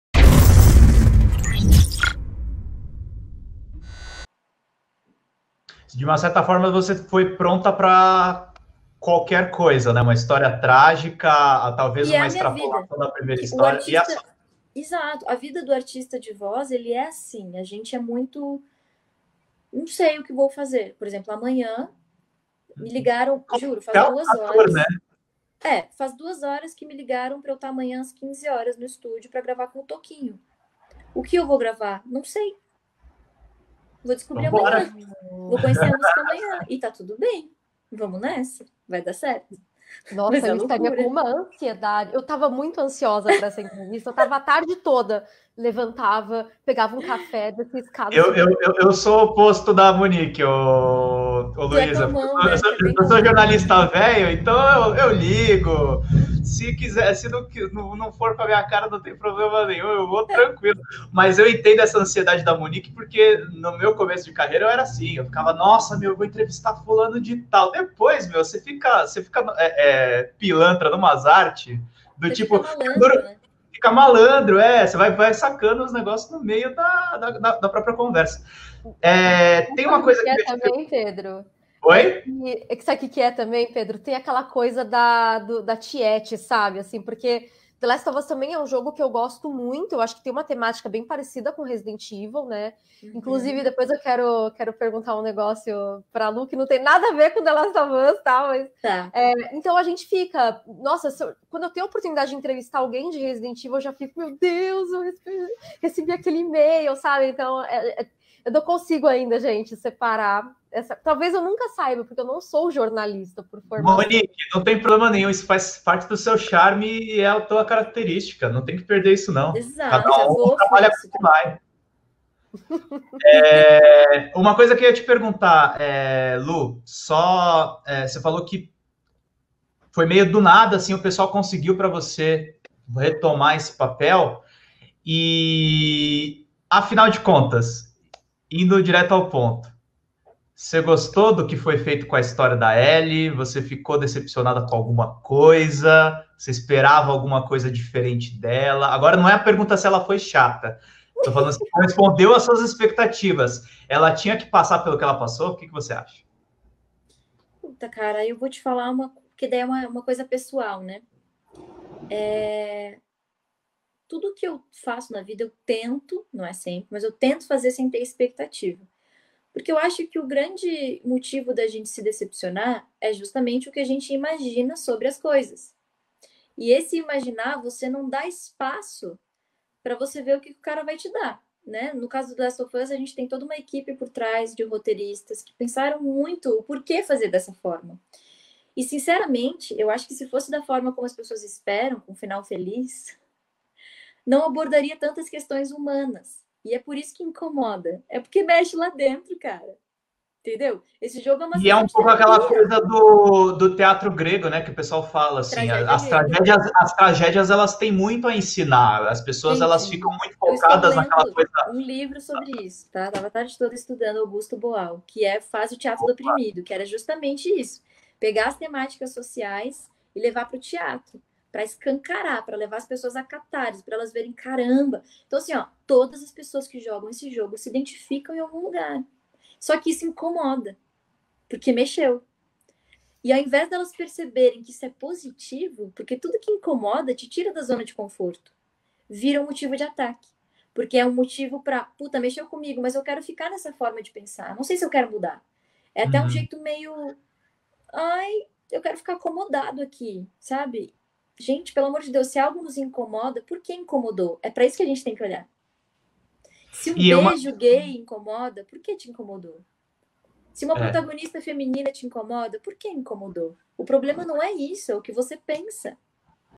CORTE: LUIZA CASPARY FALA SOBRE SER ARTISTA DE VOZ E EXPECTATIVA
Pedro Zambarda apresenta novo vídeo.

Trecho da Rádio Geek. Entrevista com a dubladora da Ellie de Last of Us 2 // NewsGames #157